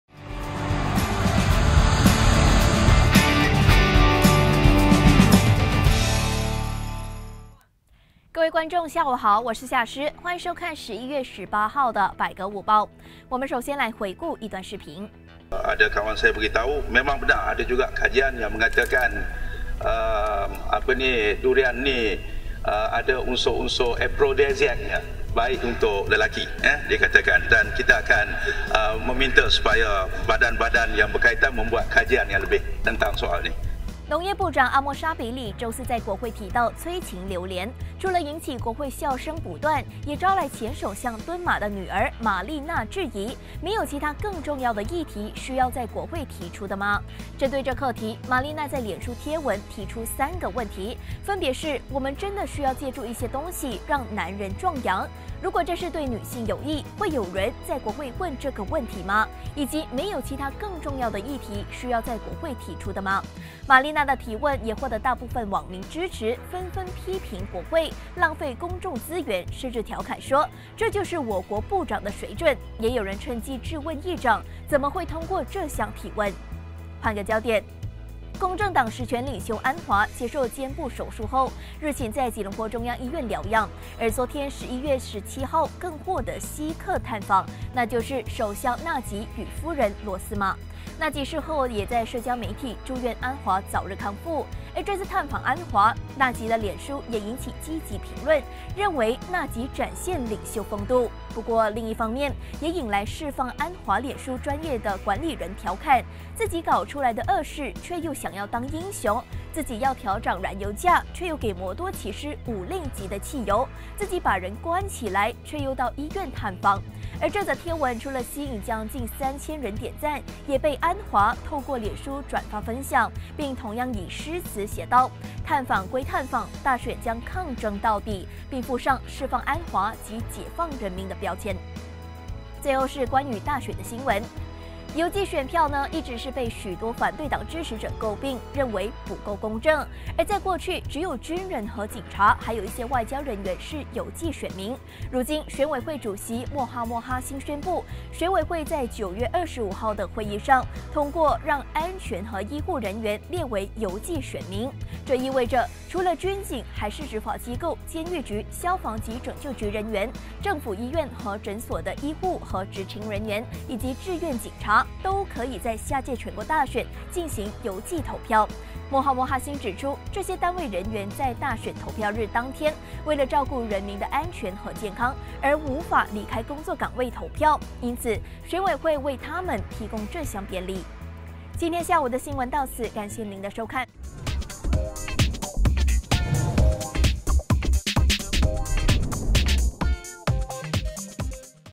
农长国会谈“催情榴莲” 玛丽娜狠批
农业部长阿末沙比里早前在国会提到“催情榴莲”，引起国会笑声不断。不过如今，前首相敦马哈迪女儿玛丽娜一连提出3个质疑……